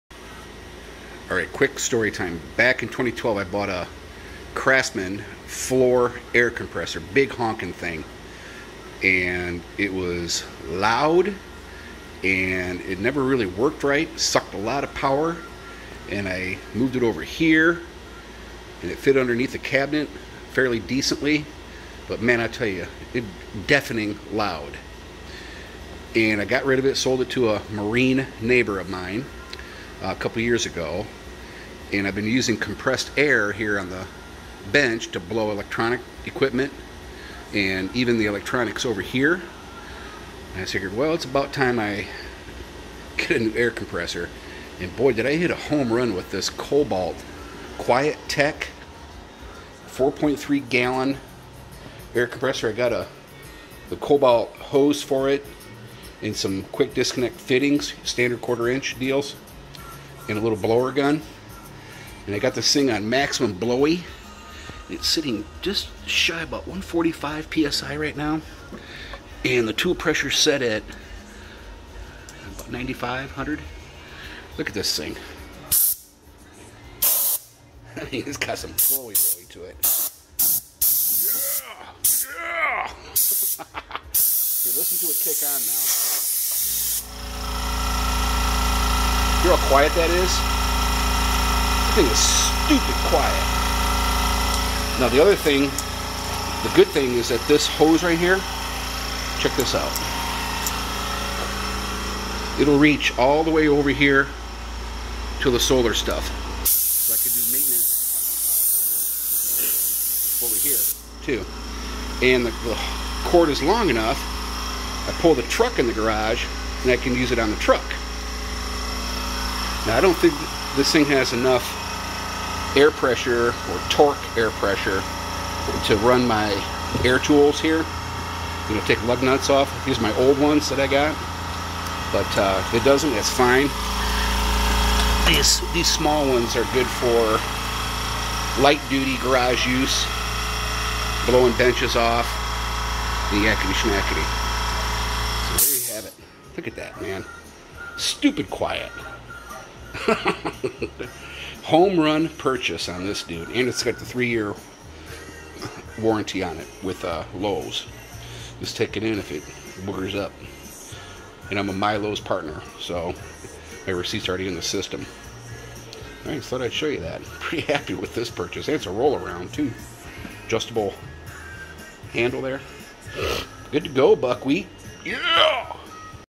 Kobalt Quiet Tech 4.3 gallon portable air compressor (from Lowe's)
Got tired of NOT having an air compressor and using canned air to blow/clean parts and electronics. I wanted something small, portable, and quiet. It also needed to have enough air delivery to use a blow gun attachment to clean parts and truck automotive work.  Hit a homerun with this Kobalt unit. I had to buy a hose, gun attachment, and some quick disconnects. Out the door...$220 for everything I bought. Win!.